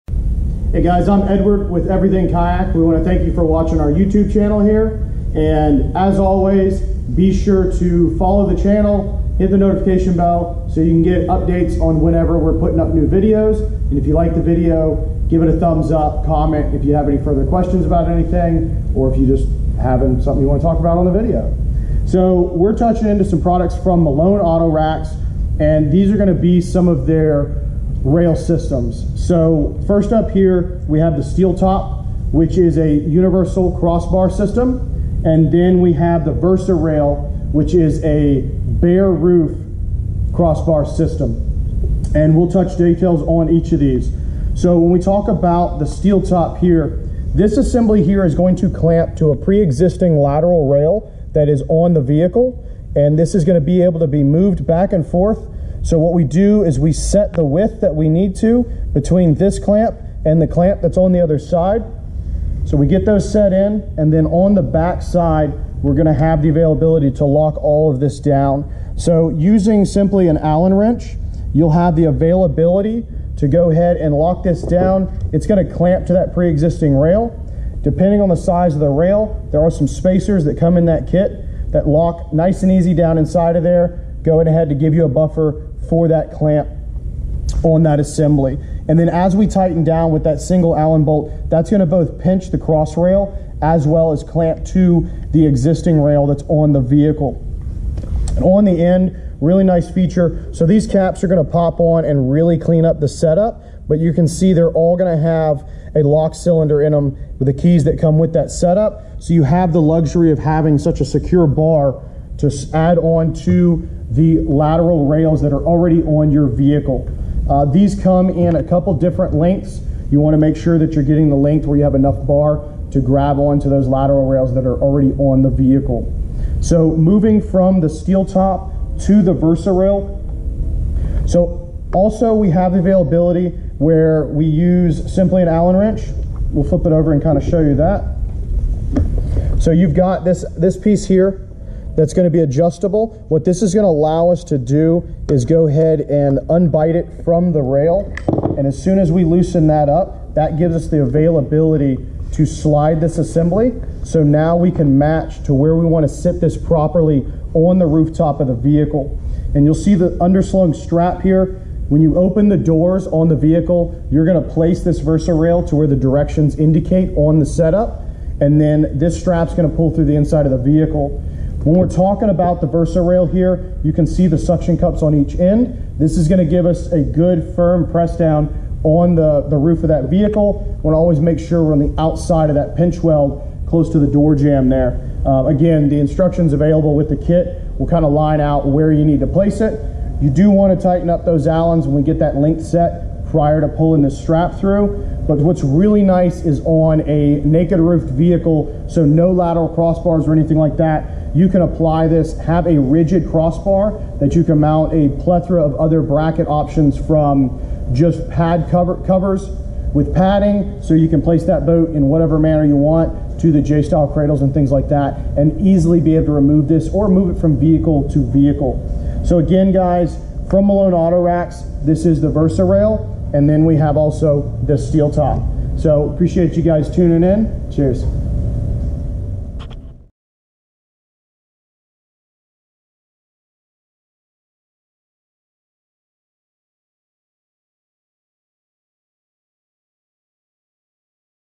Malone Auto Rack Series: Rail Systems
In this Malone Product Series you will see a break down of each of Malone's awesome roof rack options and how each of them work!
On this video we break down the Malone Rail Systems!